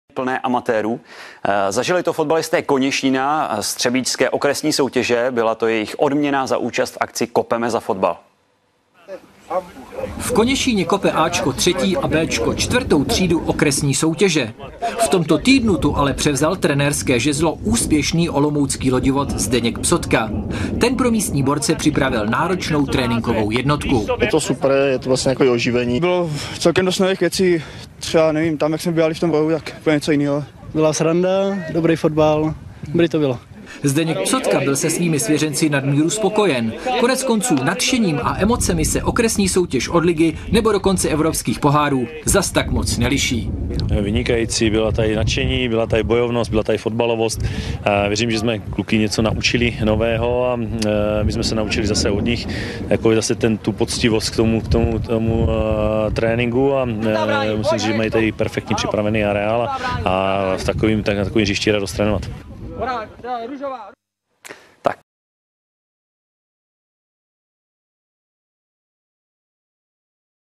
Koněšín na ČT4
Trenér Sigmy Olomouc Zdeněk Psotka trénuje fotbalisty Koněšína (7.4.2011)